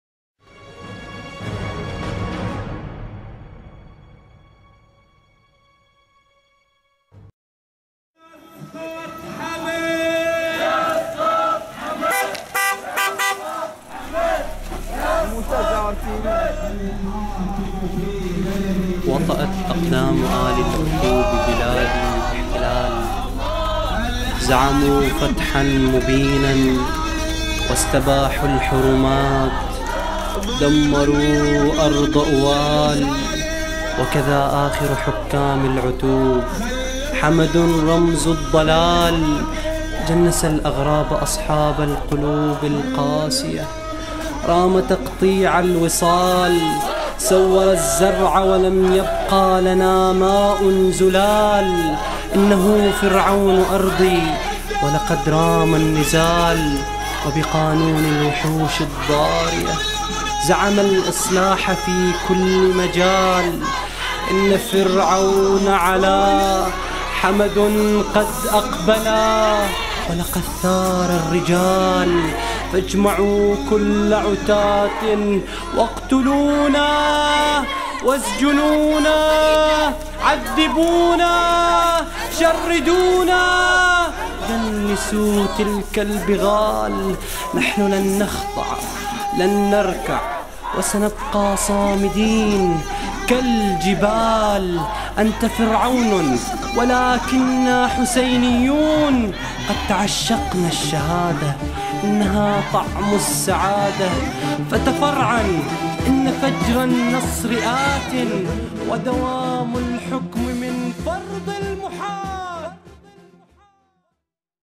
قرية القرية: مسيرة "قصتكم ممسرحة ... ليس هناك اسلحة" 2-1-2014
قرية القرية: مسيرة "قصتكم ممسرحة ... ليس هناك اسلحة" تضامُناً مع الأحبة الأسرى المعتقلين الذين تم فبركة المسرحية الهزلية ضدهم للنيل منهم 2-1-2014